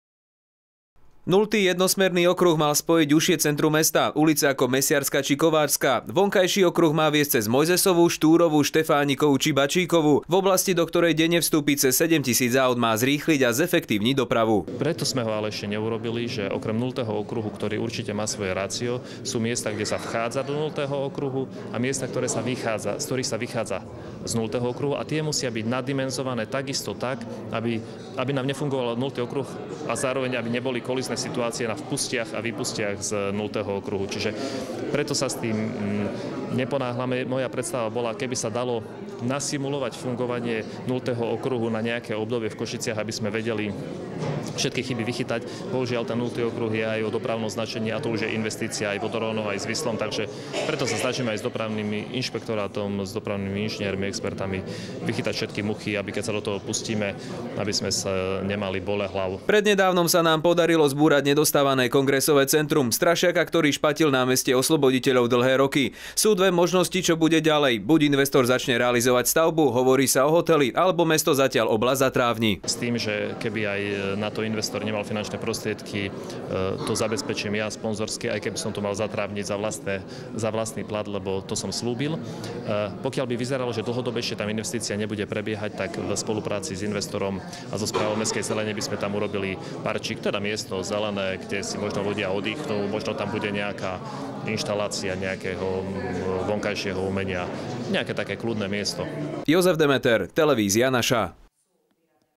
Nultý okruh v Košiciach
V nasledujúcej reportáži budeme hovoriť o tom, že jednou z vecí, ktorú mesto nestihlo vlani zrealizovať je takzvaný nultý okruh. Najväčší kruhový objazd na Slovensku  má v Košiciach zrýchliť a zefektívniť dopravu. Na druhej strane sa mesto chváli tým, že sa mu podarilo zbaviť ruiny kongresového centra na Námestí osloboditeľov. Ľudia sú zvedaví, čo na jej mieste vyrastie.